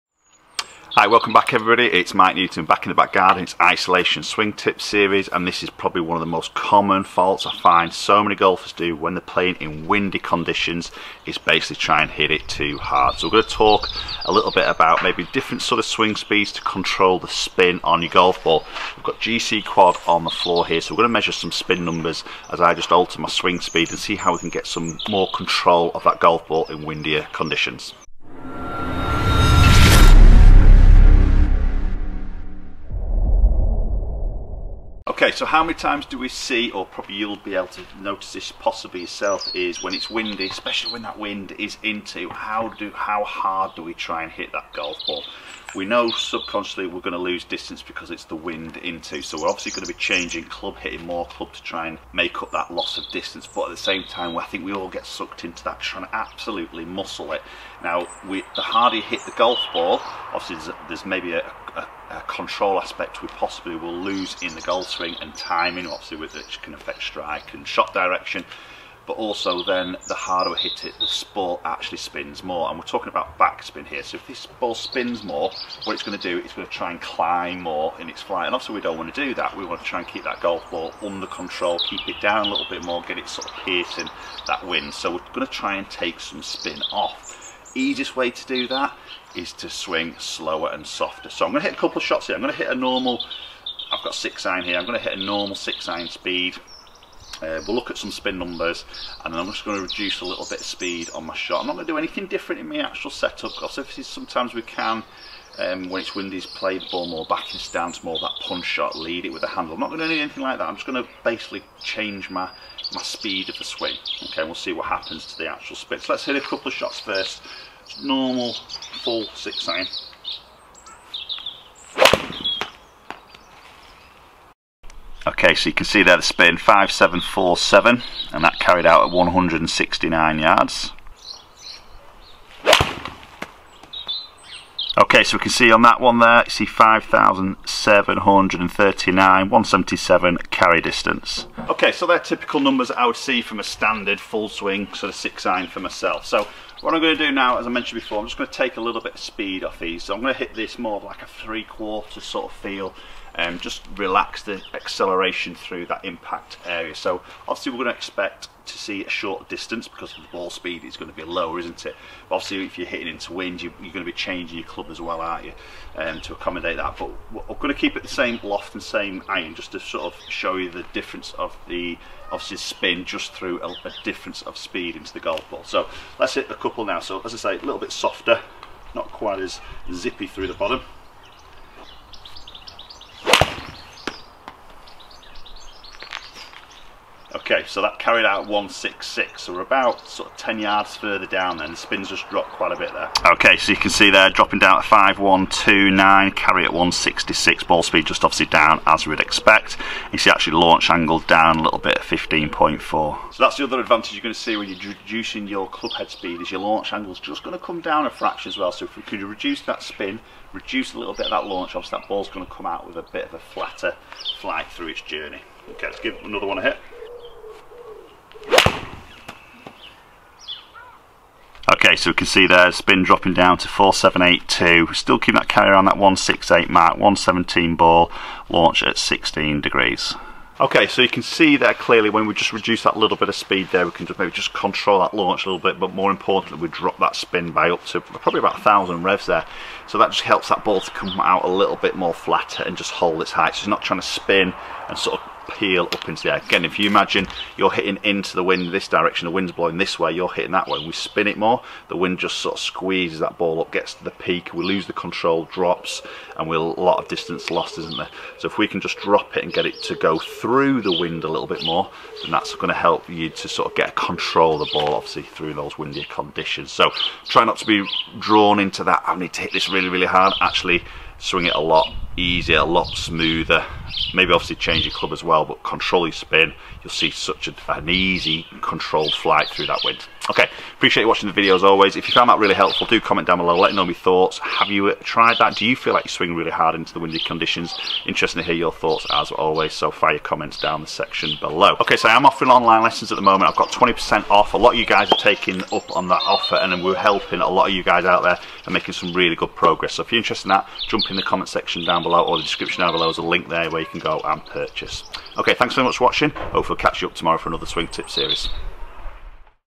How To Reduce Your Ball Spin In The Wind
In this video I provide a tip on how to gain control of your golf ball and backspin when playing in the wind.